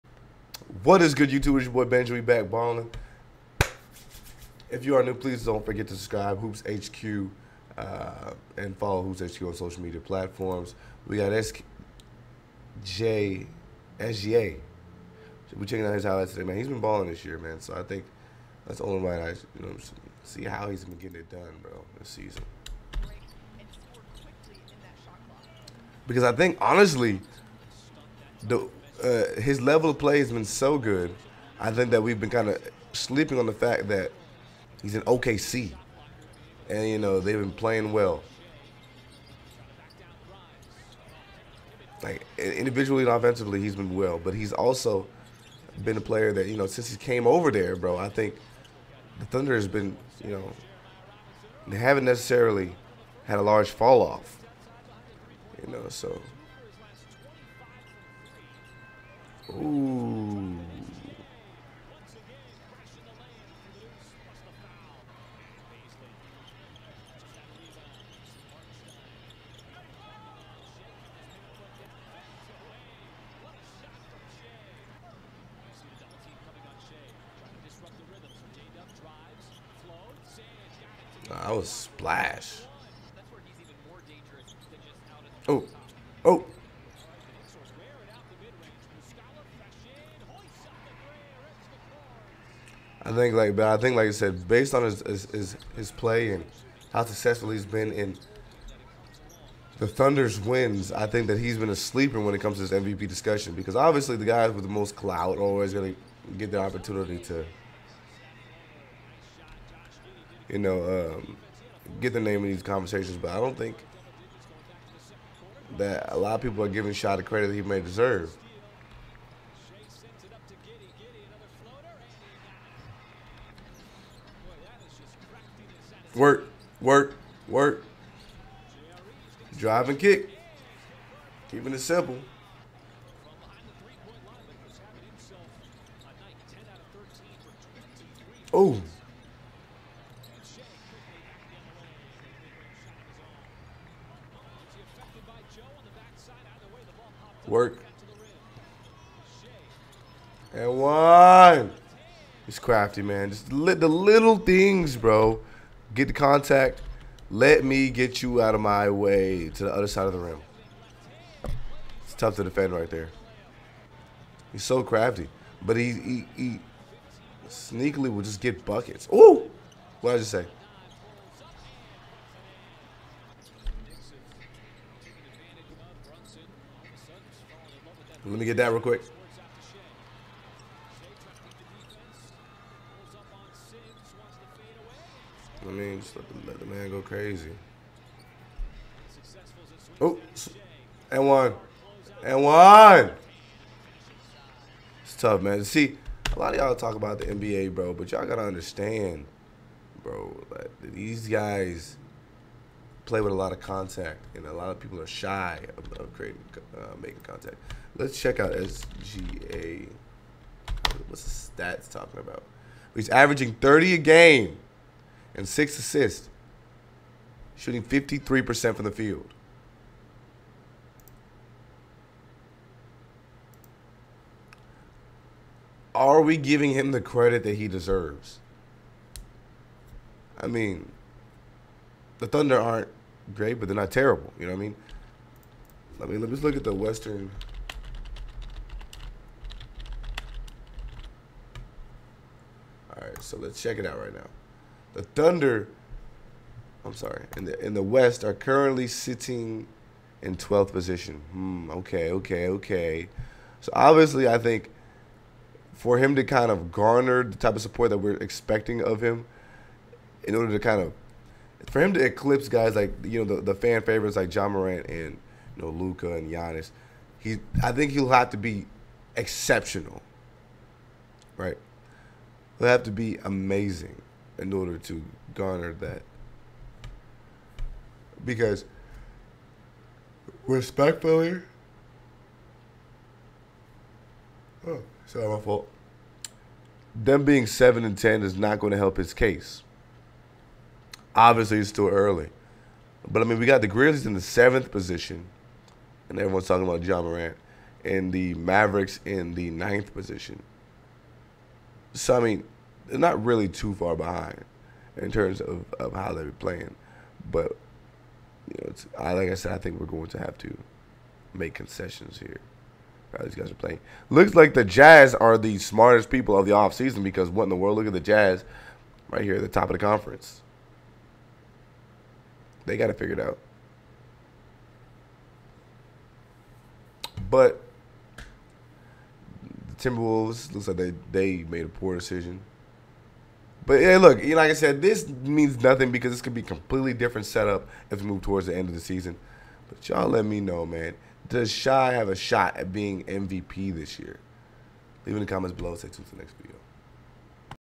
SGA FOR MVP!? 30 points Highlights vs Knicks Reaction!
Shai Gilgeous-Alexander has been putting on an offensive clinic this year, and many are claiming he should have his name in the discussion for Most Valuable Player. What do y'all think?

FOLLOW LIL BENJI!